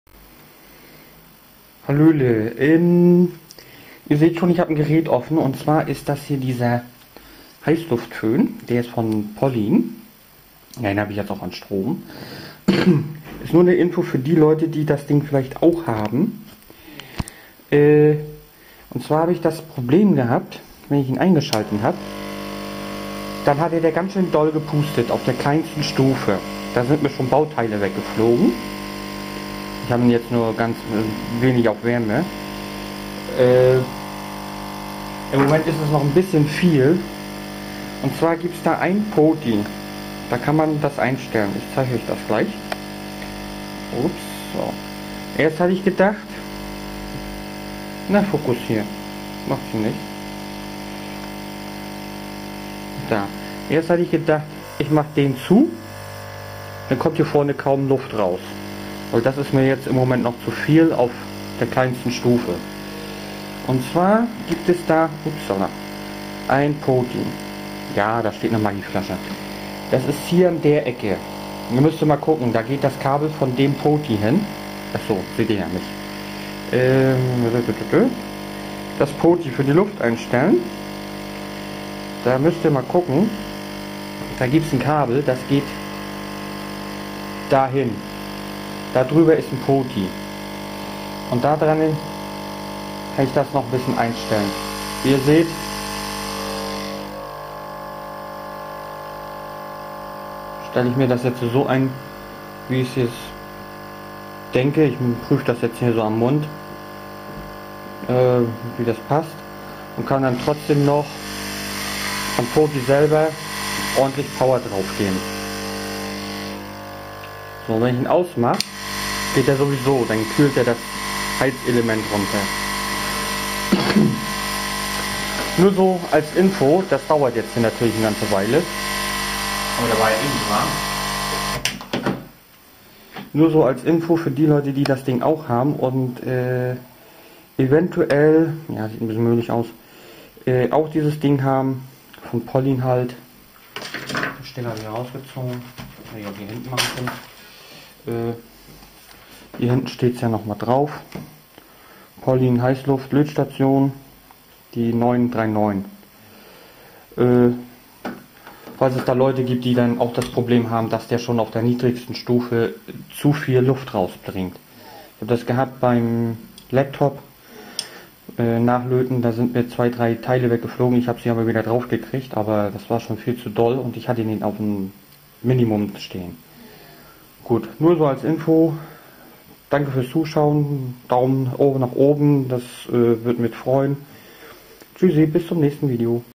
Heißluft Lötstation ZD-939 von Pollin, Luftstrom einstellen.
Hier zeige ich wie man den Luftstrom einstellen kann, wenn dieser auf niedrigster Stufe noch zu hoch sein sollte. Was bei mir der Fall war. Und da schon Bauteile in 0603 Bauform regelrecht vom Board gepustet werden.

Ich schreibe es hier nochmal hin habe es im Vid vergessen zu sagen. Bitte vor Öffnen den Netzstecker ziehen, und bei Offenem Betrieb auf die eigene Sicherheit Achten. Es sind 230V in dem Gerät vorhanden. Für Sach oder Körperliche schäden übernehme ich keine Verantwort. Jeder muss selbst wissen was er macht. Und für seine eigene Sicherheit sorgen.!